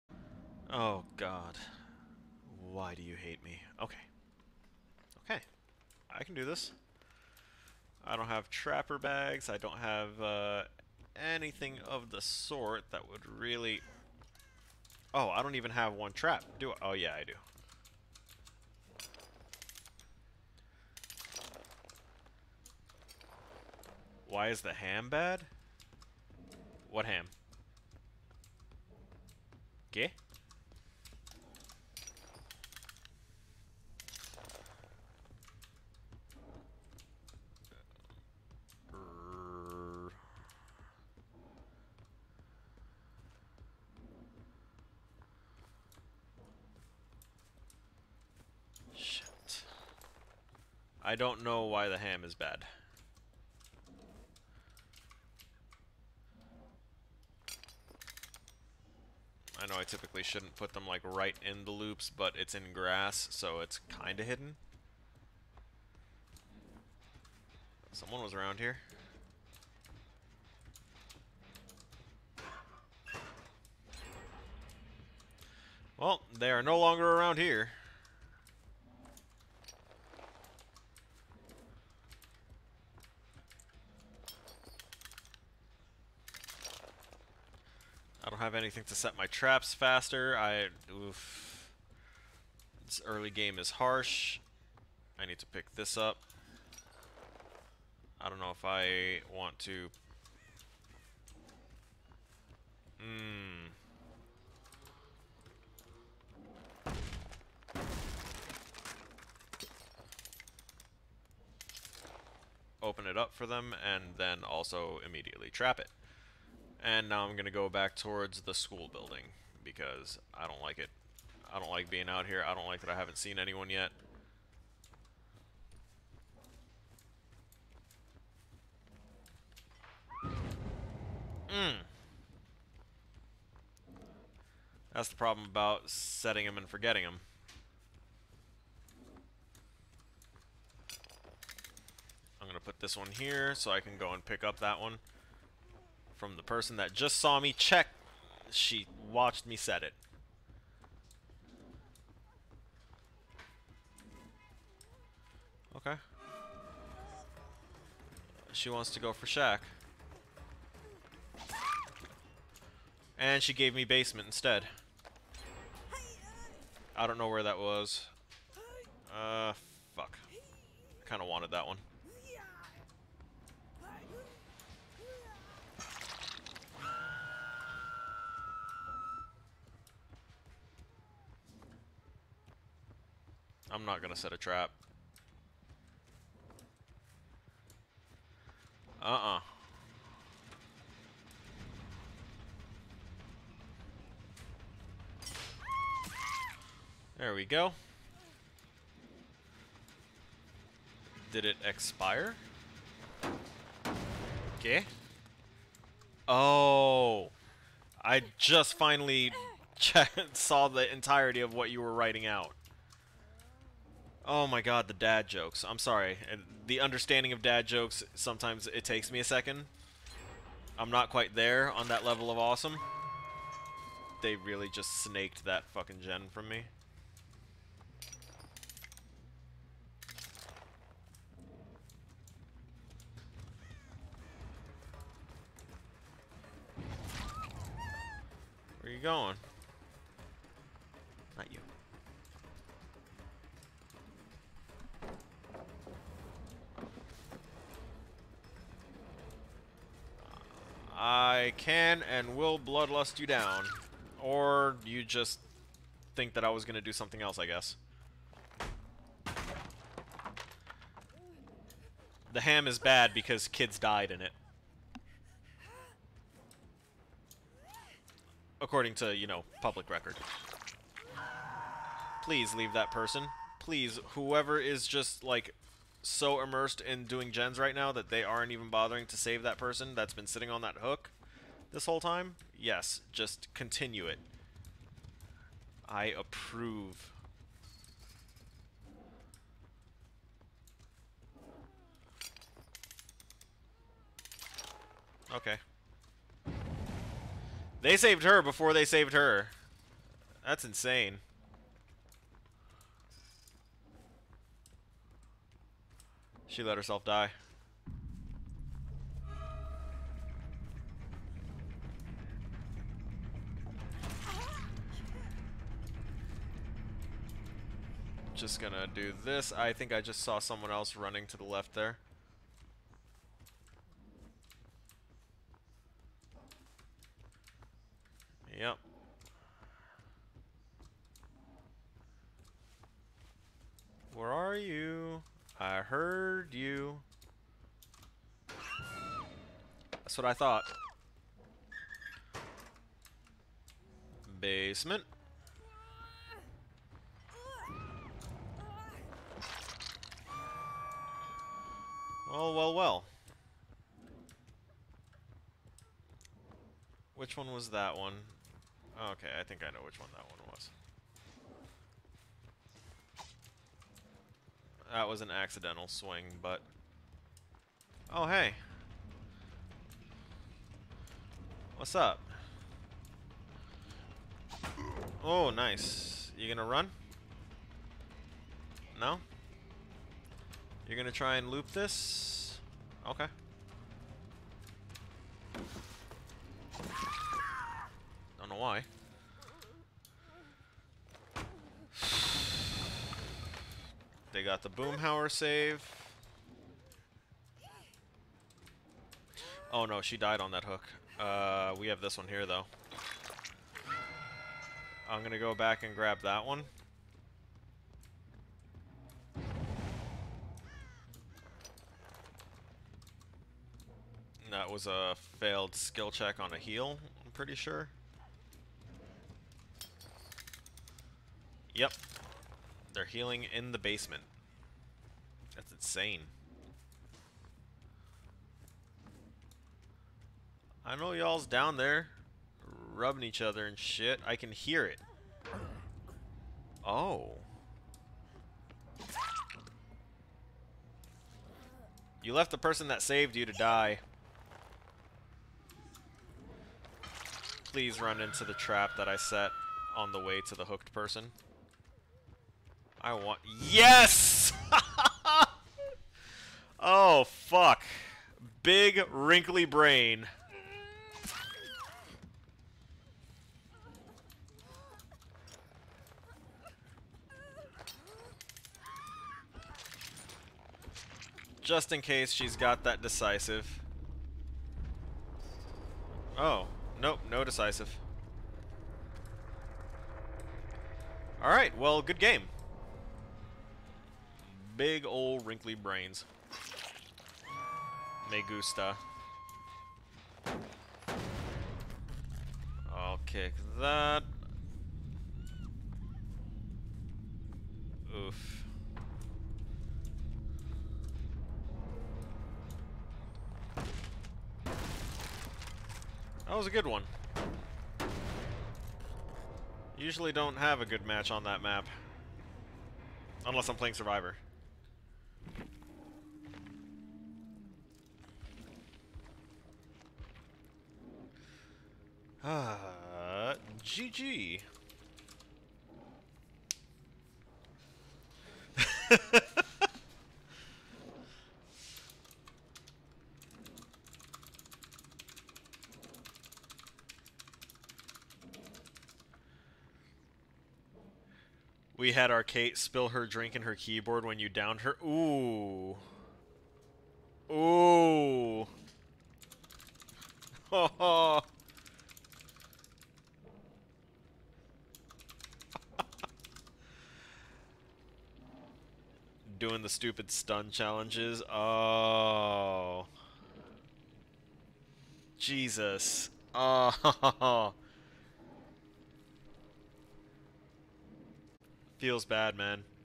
Perkless Trapper: PLANET Brain
Continuing the Perkless series, here we see the elusive trapper without any kind of gen slow down or trap speed up/ carry addons... You can start being concerned now, I certainly was.